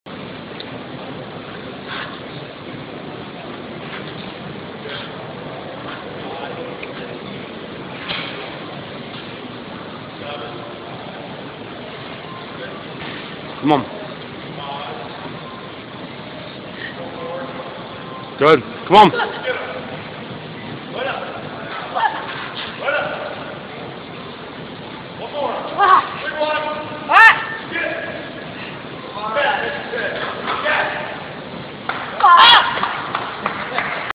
Natural Figure Pro, Nicole Weeks, February 11, 2012
10 pull-ups for my 37th birthday :)